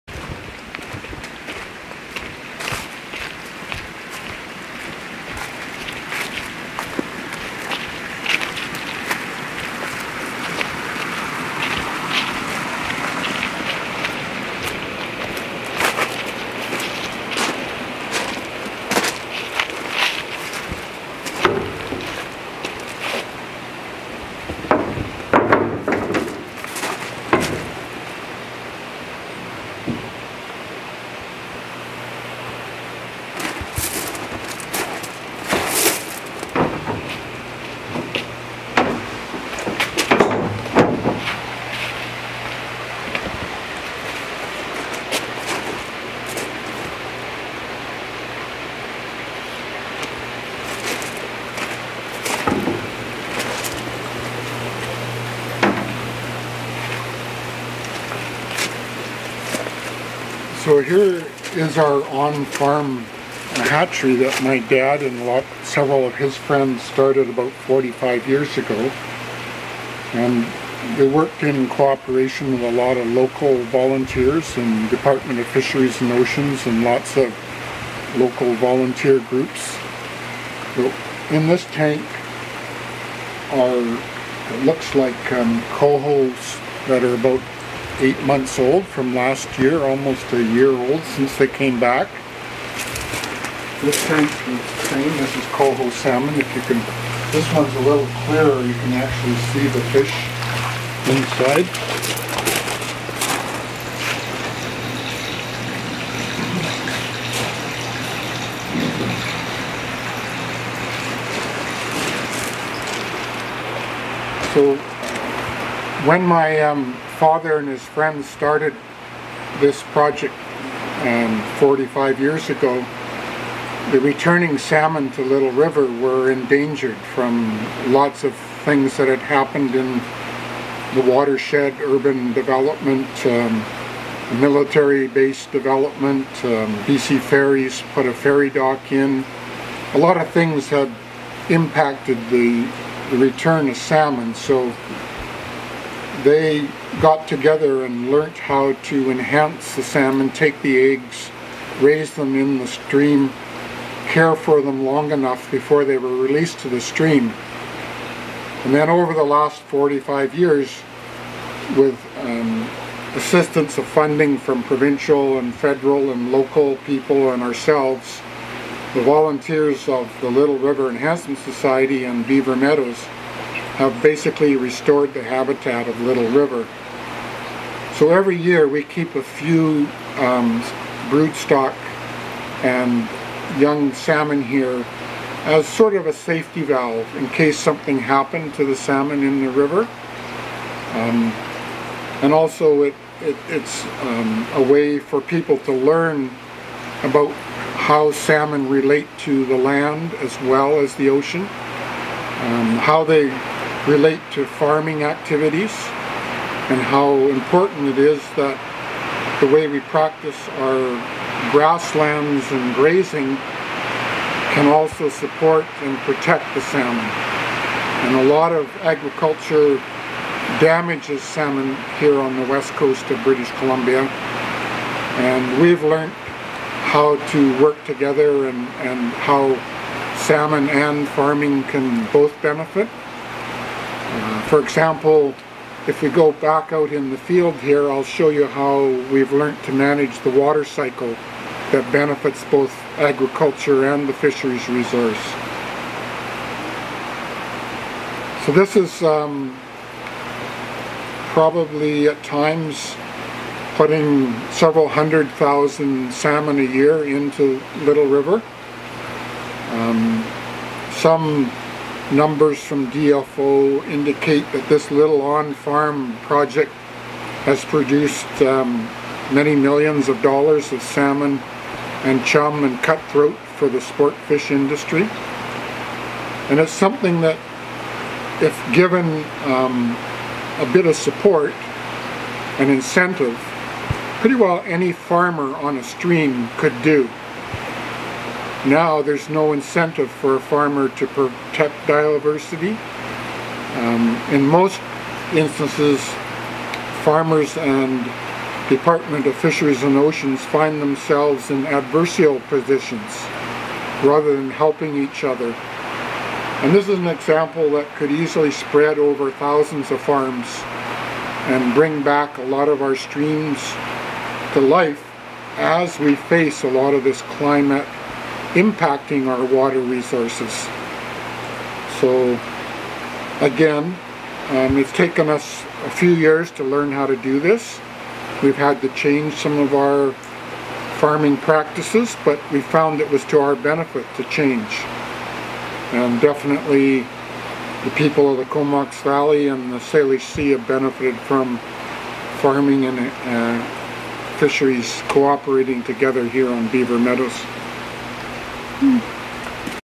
Beaver Meadow Farms Hatchery
This virtual field day features Edgar Smith from Beaver Meadow Farms who takes us on a farm tour and talks about the ways they practice climate-friendly agriculture. This video is specific to talking about the hatchery, the rest of the farm tour is in a previous video.